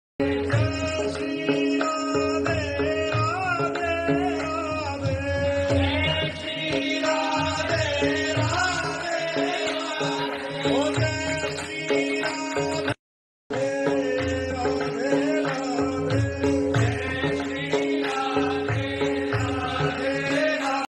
planting of tree give too much fun its my hobby🙏its easiest type of yagn in kaliyug
planting of tree give too much fun its my hobby🙏